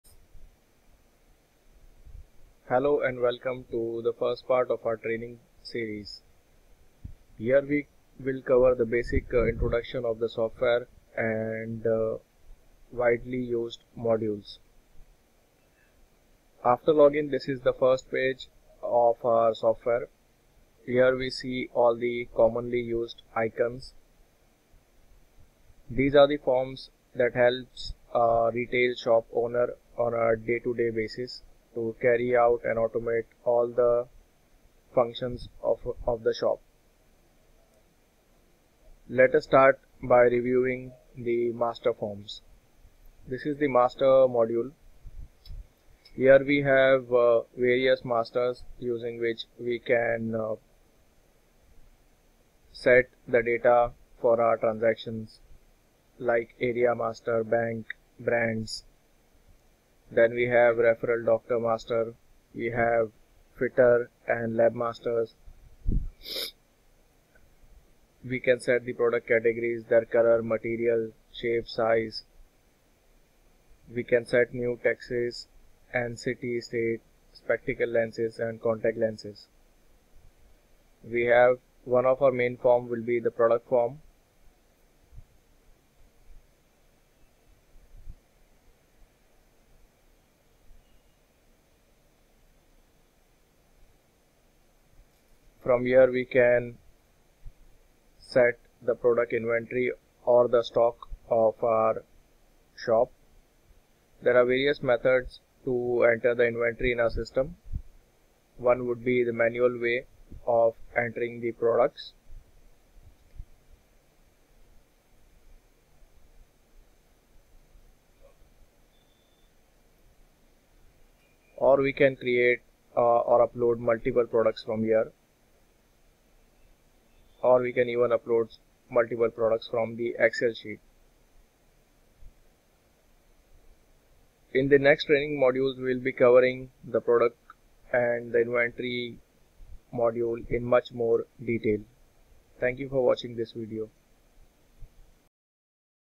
Overview of Optical POS Software
This video contains basic overview of optical software.